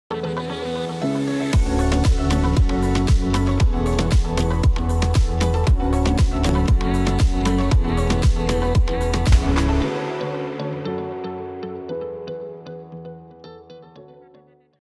Quicksilver 675 Cruiser
Active 675 Cruiser aptakus dizainas derinamas su sportiška konstrukcija ir puikiu erdvės išplanavimu. Čia tilps iki 7 žmonių, mėgstančių plaukioti kruizu ir mėgautis saule.
Garant Boats & Yachts yra oficialus Quicksilver katerių atstovas Lietuvoje.